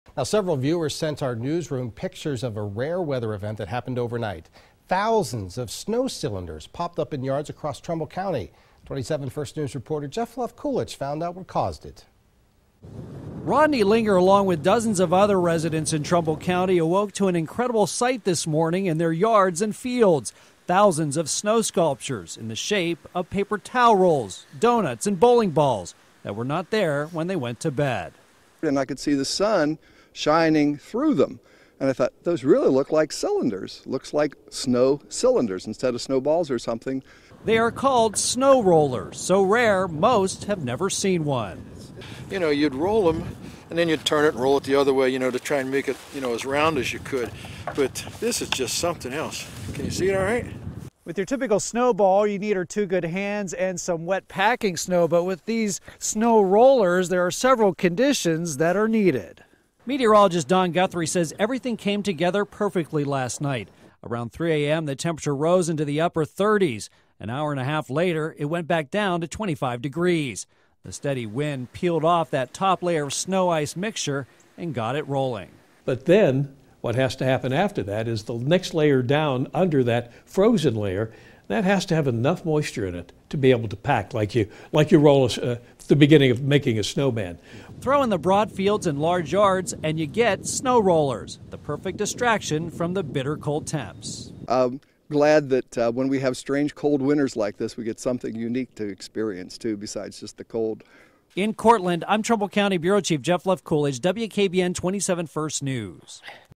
Frigid air causes rare occurrences
Jeff Levkulich explains the weather phenomenon of snow rollers.